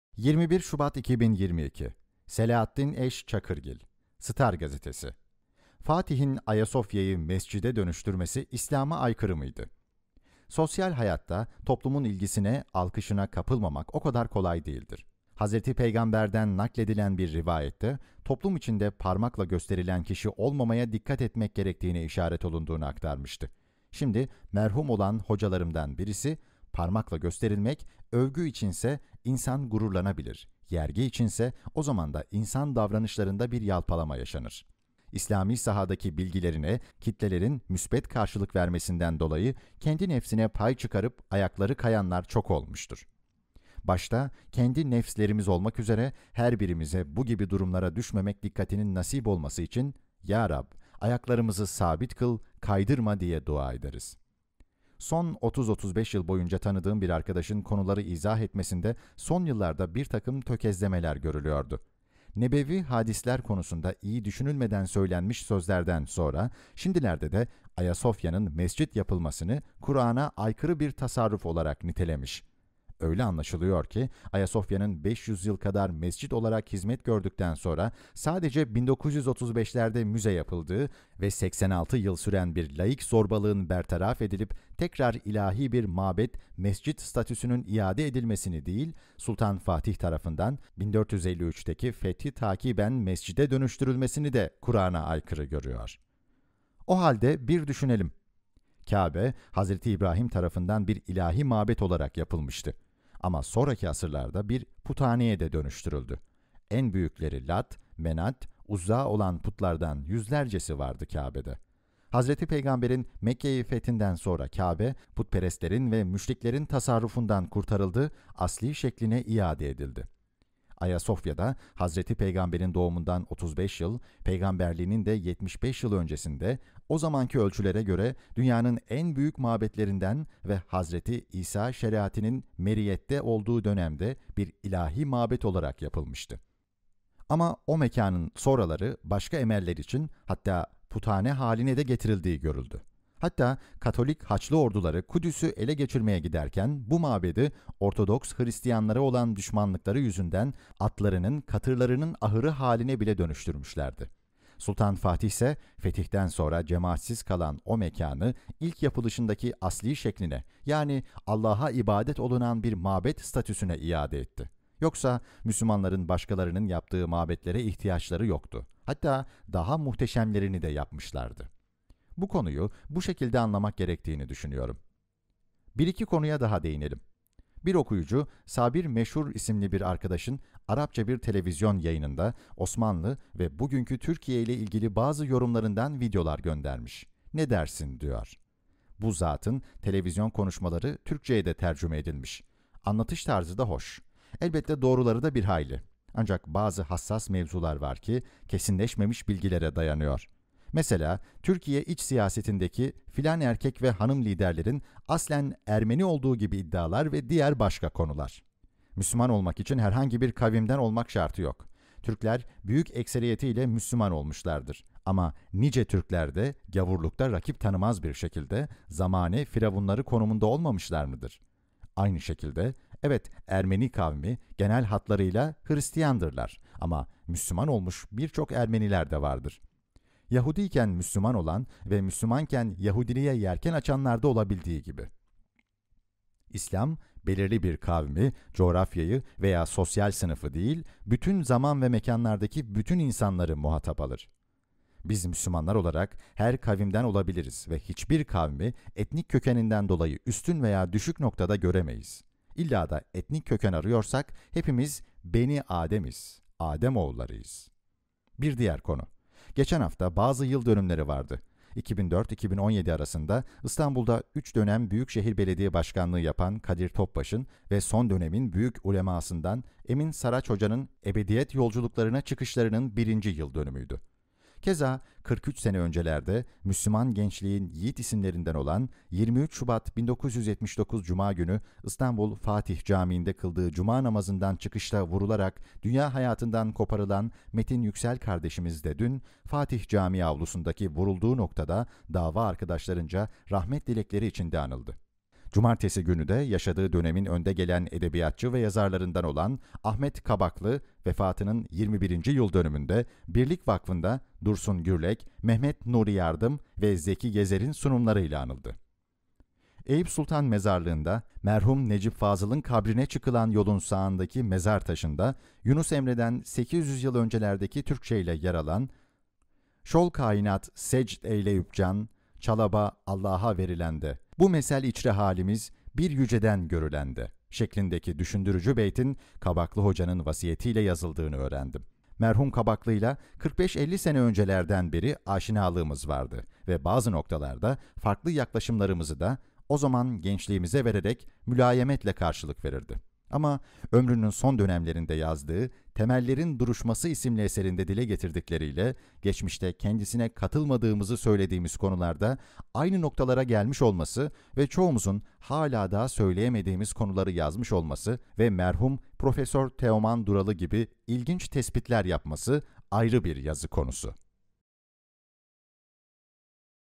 Fatih'in Ayasofya'yı ‘mescid'e dönüştürmesi', İslâm'a aykırı mıydı? - Selahaddin E. Çakırgil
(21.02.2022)

Sosyal hayatta, toplumun ilgisine, alkışına kapılmamak, o kadar kolay değildir.

Hz. Peygamber (S)'den nakledilen bir 'rivayet'te, 'toplum içinde parmakla gösterilen kişi olmamaya dikkat etmek gerektiği'ne işaret olunduğunu aktarmıştı; şimdi merhûm olan 'hoca'larımdan birisi... 'Parmakla gösterilmek', övgü için ise, insan gururlanabilir; yergi içinse, o zaman da insan davranışlarında bir yalpalama yaşanır.

İslâmî sahadaki bilgilerine kitlelerin müsbet karşılık vermesinden dolayı, kendi nefsine pay çıkarıp ayakları kayanlar çok olmuştur.

Başta kendi 'nefs'lerimiz olmak üzere, her birimize, bu gibi durumlara düşmemek dikkatinin nasib olması için, 'Yâ Rab! Ayaklarımızı sâbit kıl, kaydırma!' diye dua ederiz.

Son 30-35 yıl boyunca tanıdığım bir arkadaşın konuları izah etmesinde son yıllarda bir takım tökezlemeler görülüyordu. 'Nebevî hadisler' konusunda iyi düşünülmeden söylenmiş sözlerinden sonra, şimdilerde de, 'Ayasofya'nın mescid yapılmasını Kur'an'a aykırı bir tasarruf olarak nitelemiş... Öyle anlaşılıyor ki, Ayasofya'nın 500 yıl kadar mescid olarak hizmet gördükten sonra, sadece 1935'lerde müze yapıldığı ve 86 yıl süren bir laik zorbalığın bertaraf edilip, tekrar ilahî bir mâbed / mescid statüsünün iade edilmesini değil, Sultan Fatih tarafından -1453'deki fethi takiben- mescid'e dönüştürülmesini de 'Kur'an'a aykırı' görüyor.

O halde bir düşünelim...

Kâbe, Hz. İbrahîm tarafından bir ilâhî mâbed olarak yapılmıştı. Ama, sonraki asırlarda bir puthaneye de dönüştürüldü. En büyükleri Lât, Menât, Uzzâ olan putlardan yüzlercesi vardı, Kâbe'de... Hz. Peygamber (S)'in Mekke'yi fethinden sonra, Kâbe, putperestlerin ve müşriklerin tasarrufundan kurtarıldı, aslî şekline iade edildi.

Ayasofya da, Hz. Peygamber'in doğumundan 35 yıl, peygamberliğinin de 75 yıl öncesinde, o zamanki ölçülere göre, dünyanın en büyük mâbedlerinden ve Hz. İsâ şeriatinin mer'iyette olduğu dönemde bir ilâhî mâbed olarak yapılmıştı.

Ama, o mekânın sonraları, başka emeller için, hattâ puthane haline de getirildiği görüldü. Hattâ, Katolik Haçlı Orduları Kudüs'e ele geçirmeye giderken, bu mâbedi, Ortodoks Hristiyanlar'a olan düşmanlıkları yüzünden, atlarının, katırlarının ahırı haline bile dönüştürmüşlerdi.

Sultan Fatih ise, fetihten sonra cemaatsiz kalan o mekânı, ilk yapılışındaki aslî şekline, yani Allah'a ibadet olunan bir mâbed statüsüne iade etti... Yoksa, Müslümanların başkalarının yaptığı mâbedlere ihtiyaçları yoktu; hattâ daha muhteşemlerini de yapmışlardı.

Bu konuyu, bu şekilde anlamak gerektiğini düşünüyorum.

Bir-iki konuya da değinelim...

Bir okuyucu, Sâbir Meşhûr isimli bir arkadaşın Arabça bir tv. yayınında, Osmanlı ve bugünkü Türkiye'yle ilgili bazı yorumlarından videolar göndermiş, 'Ne dersin?' diyor...

Bu zâtın tv. konuşmaları Türkçeye de tercüme edilmiş... Anlatış tarzı da hoş... Elbette doğruları da bir hayli... Ancak, bazı hassas mevzular var ki, kesinleşmemiş bilgilere dayanıyor. Meselâ, Türkiye iç siyasetindeki filân erkek ve hanım liderlerin aslen 'Ermeni olduğu' gibi iddialar ve diğer başka konular...

Müslüman olmak için herhangi bir kavimden olmak şartı yok... Türkler büyük ekseriyetiyle Müslüman olmuşlardır, ama, nice Türkler de gâvurlukta rakip tanımaz bir şekilde, zamâne firavunları konumunda olmamışlar mıdır? Aynı şekilde, evet Ermeni kavmi, genel hatlarıyla Hristiyan'dırlar; ama, Müslüman olmuş bir çok Ermeniler de vardır. Yahudi'yken Müslüman olan ve Müslüman iken Yahudiliğe yelken açanlar da olabildiği gibi... İslâm, belirli bir kavmi, coğrafyayı veya sosyal sınıfı değil, bütün zaman ve mekânlardaki bütün insanları muhatab alır.

Biz Müslümanlar olarak, her kavimden olabiliriz ve hiçbir kavmi, etnik kökeninden dolayı üstün veya düşük noktada göremeyiz... İllâ da etnik köken arıyorsak, hepimiz, 'Benî Âdem'iz, / Âdem oğullarıyız...'

Bir diğer konu...

Geçen hafta, bazı yıldönümleri vardı... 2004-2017 arasında İstanbul'da 3 dönem Büyükşehir Belediye Başkanlığı yapan Kadir Topbaş'ın ve son dönemin büyük ulemâ'sından M. Emin Saraç Hoca'nın ebediyet yolculuklarına çıkışlarının birinci yıldönümüydü.

Kezâ, 43 sene öncelerde, Müslüman gençliğin yiğit isimlerinden olan, 23 Şubat 1979 Cuma günü, İstanbul-Fatih Camii'nde kıldığı Cuma namazından çıkışta vurularak dünya hayatından koparılan Metin Yüksel kardeşimiz de dün, Fatih Câmii avlusundaki vurulduğu noktada dâva arkadaşlarınca rahmet dilekleri içinde anıldı.

Cumartesi günü de, yaşadığı dönemin önde gelen edebiyatçı ve yazarlarından olan Ahmed Kabaklı (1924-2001), vefatının 21. Yıldönümünde, Birlik Vakfı'nda, Dursun Gürlek, Mehmed Nurî Yardım ve Zeki Gezer'in sunumlarıyla anıldı.